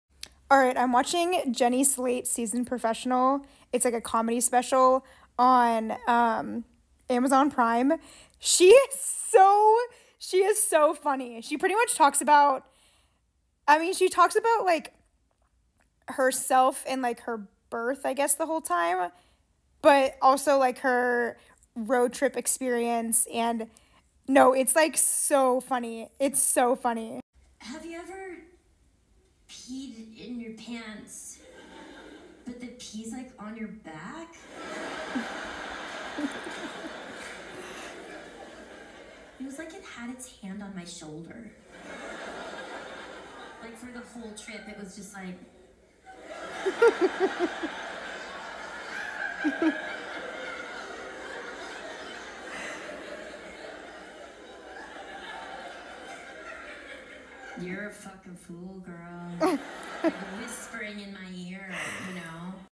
Jenny Slate: Seasoned Professional Comedy Special Review
The new Amazon prime Jenni, Slate: seasoned professional, comedy special is so funny! You have to watch it! 10 out of 10 recommend .